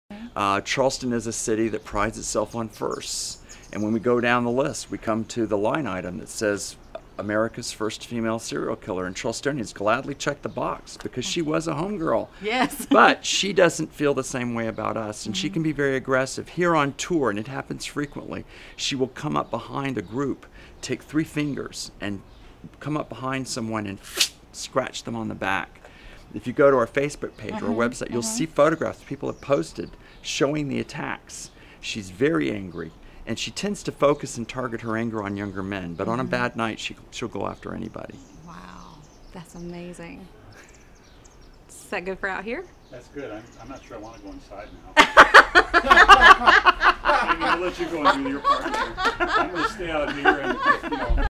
Have You Been Marked by Lavinia Fisher?
Here's another piece of our interview with Randy Johnson of Bulldog Tours in Charleston, South Carolina. In this one, he talks about Lavinia's favorite thing to do to make herself known as she haunts the Old City Jail. Stick around to the end, and you'll see a candid moment where our videographer was creeped out by the story!